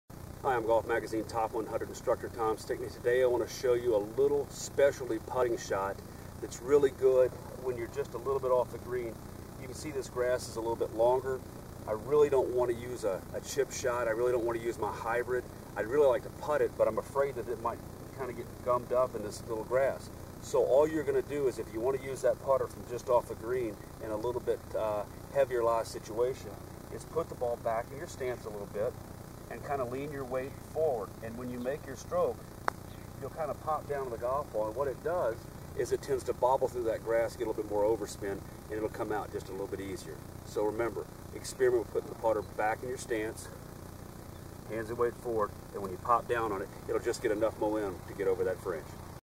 Special Texas Wedge Shot with Putter
Try this shot when putting off the green just won't work